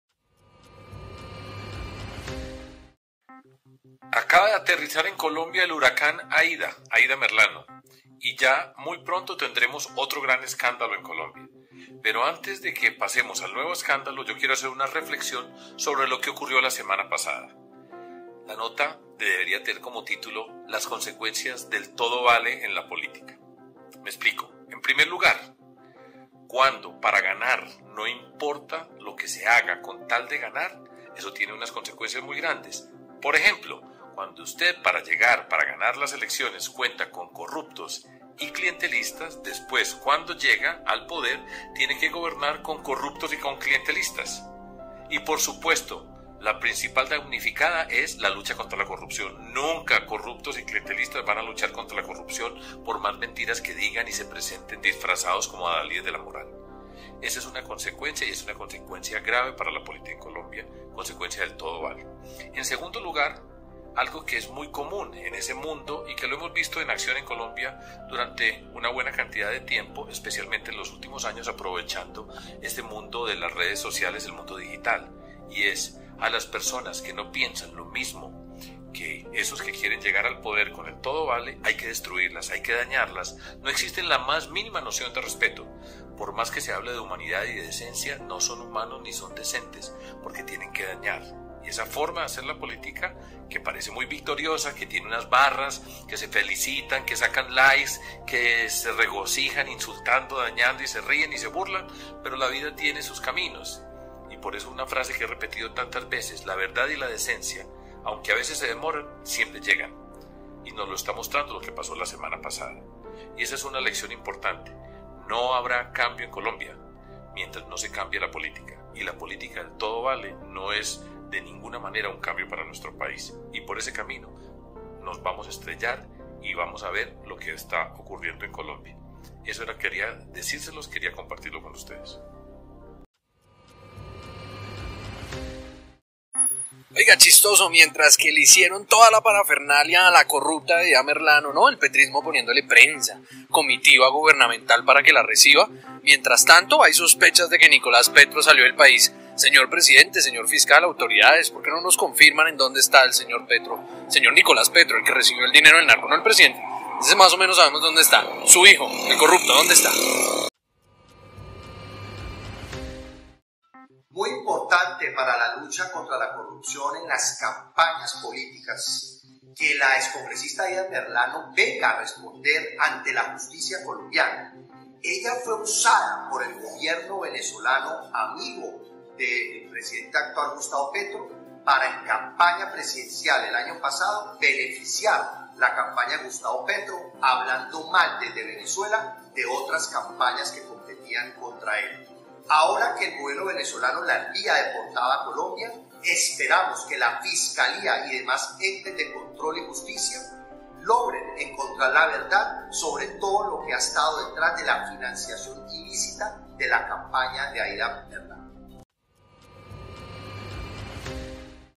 🔴 LLEGÓ AIDA MERLANO Y SE VOLÓ NICOLAS
Sergio Fajardo
Nicolas Gómez Arenas
Christian Garces
El día del temblor llegó el huracán Aida Merlano y pronto tendremos un nuevo escándalo. Antes de que le echen tierra a lo que pasó la semana pasada, quiero compartir un par de lecciones que nos deja la política del TODO VALE. Esto apenas comienza y todavía faltan mucho por ver.